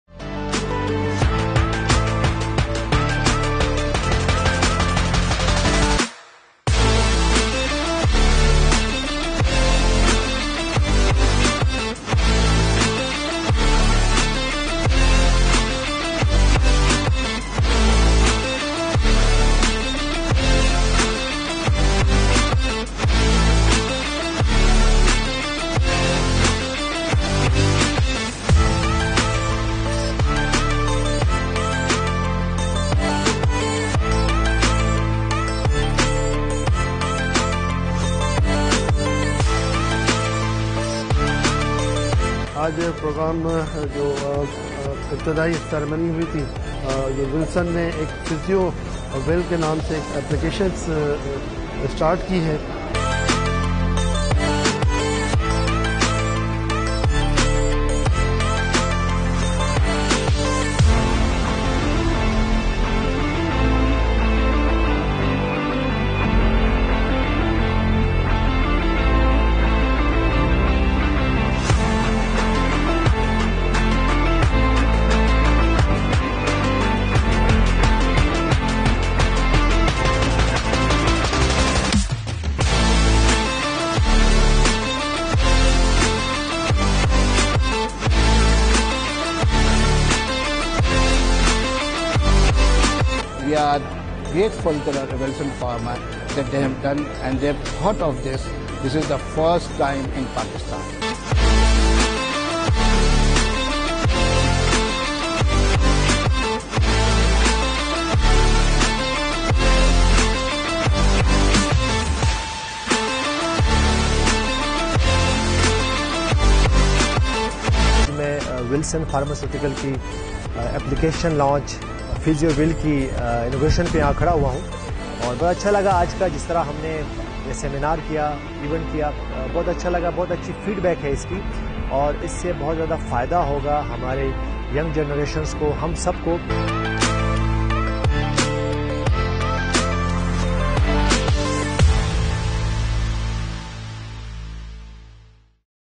Another remarkable PhysioWil launch event held in Karachi on 26th Aug’23.
Watch the video now and be a part of the movement that's transforming the way we approach physiotherapy.
The event was graced by the presence of the legendary cricketer, Yunus Khan, as the brand ambassador of PhysioWil. His presence added an extra touch of elegance and significance to the occasion.
Anchor cum moderator, Dr. Muzna Ebrahim, skillfully guided us through the event, setting the stage for an unforgettable evening. We were also honored to have Dr. Mujahid Jamil Khattak (AKUH) as a speaker, sharing invaluable wisdom and expertise with the audience. His contributions further enriched the event, inspiring all who attended.
The panel of experts included renowned personalities such as Prof. Parwez Anjum; Prof. Nusrat Rasheed; Prof. Akhter Baig; Prof. Muhammad Ali. Their profound knowledge and insights into healthcare revolutionized the way we perceive and approach physiotherapy.
Join us in shaping a healthier and more vibrant world with PhysioWil!